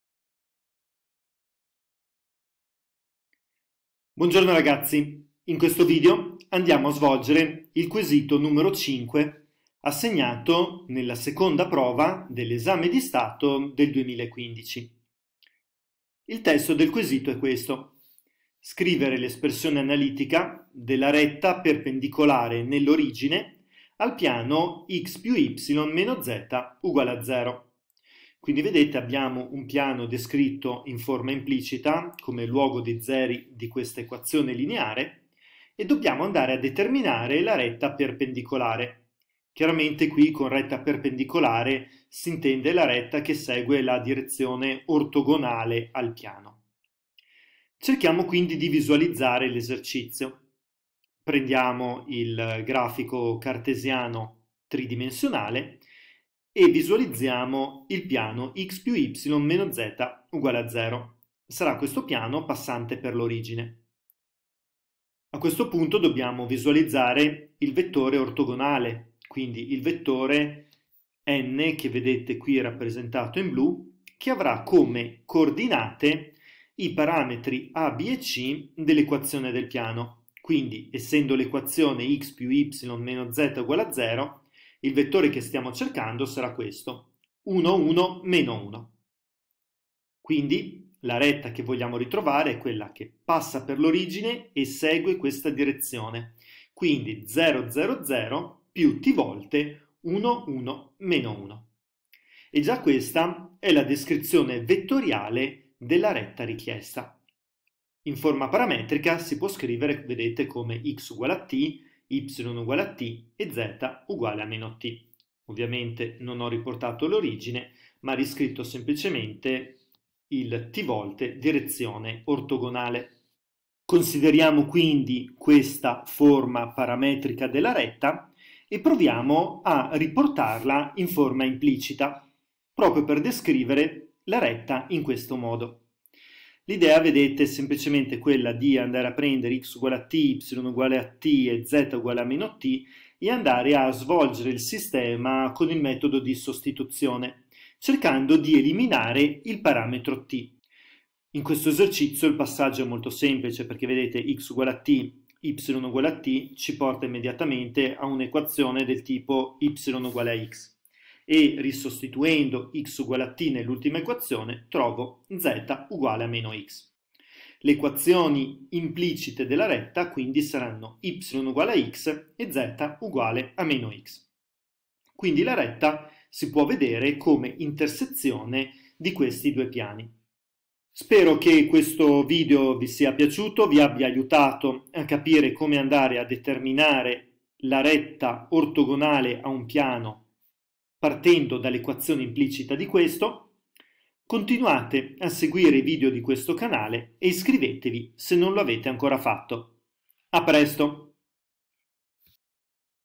Retta ortogonale a un piano, quesito 5 esame 2015
Retta ortogonale a un piano, quesito 5 esame 2015: risoluzione del quesito 5 della seconda prova d'esame di stato (esame di maturità) del 2015: retta ortogonale (perpendicolare) a un piano passante per un punto, descritta in forma parametrica e in forma implicita (luogo di zeri)

Se il video ti è piaciuto, metti il MI PIACE e lascia un COMMENTO!